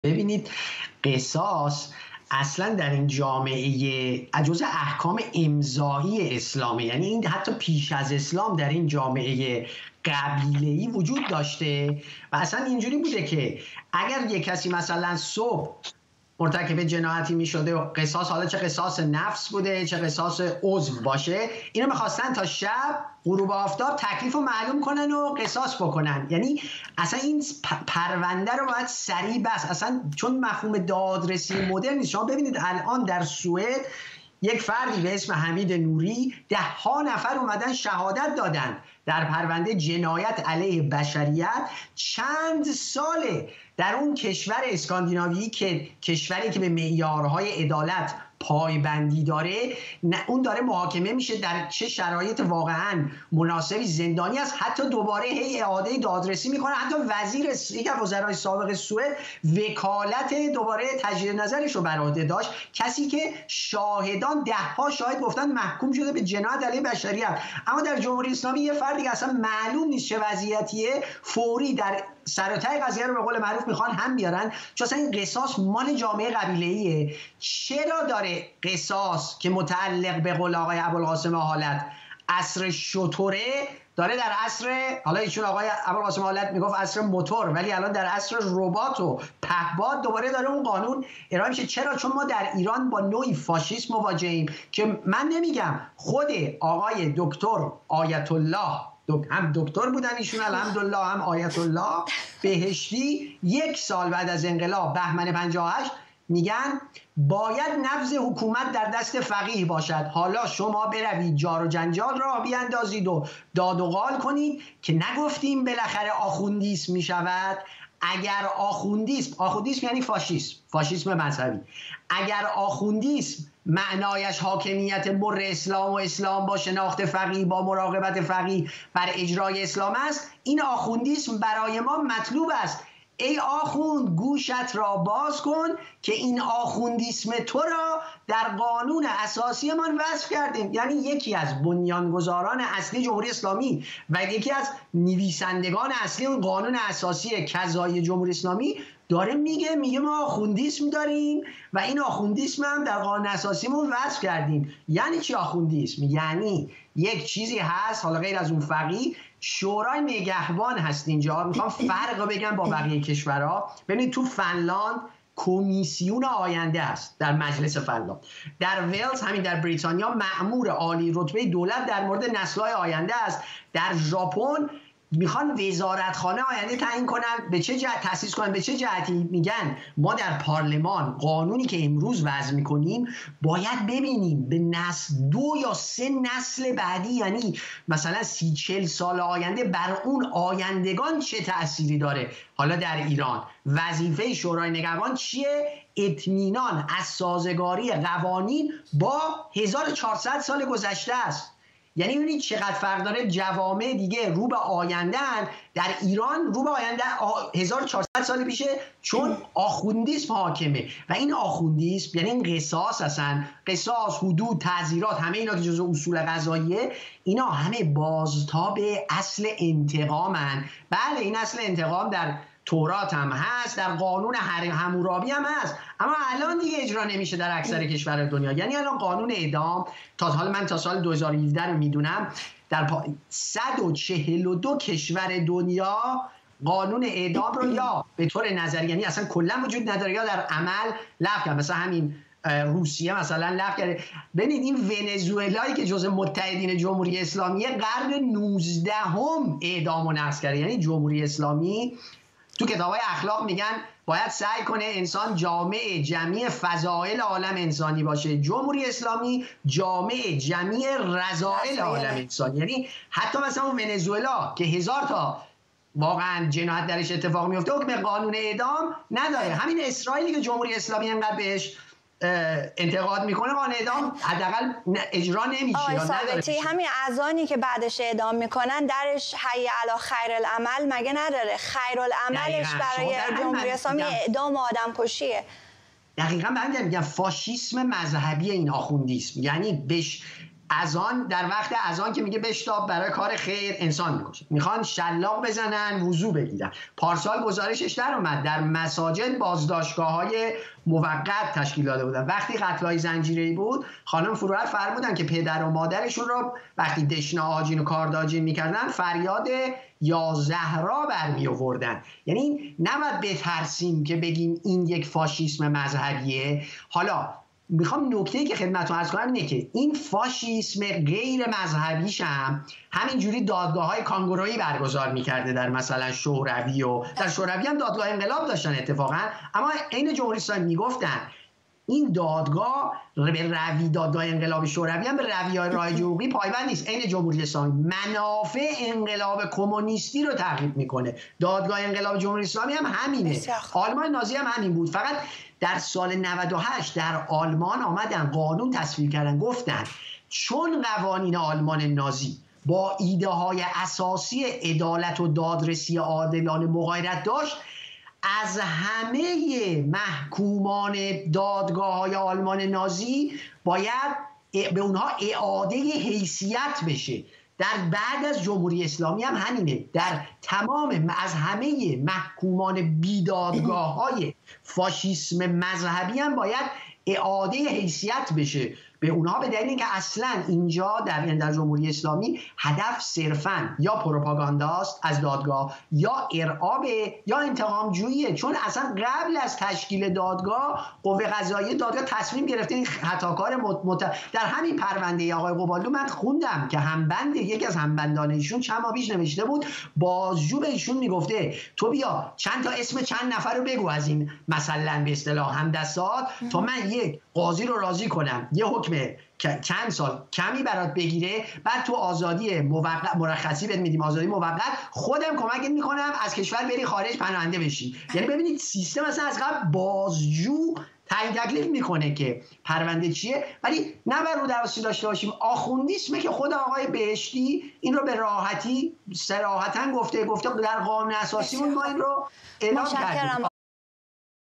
آخوندیسم، قصاص و دست باز جمهوری اسلامی برای قتل شهروندان
عرفان ثابتی، جامعه‌شناس، می‌گوید که جمهوری اسلامی با استناد به قوانین یک‌هزار و چهار سال پیش و با اتکا به قصاص می‌تواند به راحتی شهروندان را ایران بکشد.
چشم‌انداز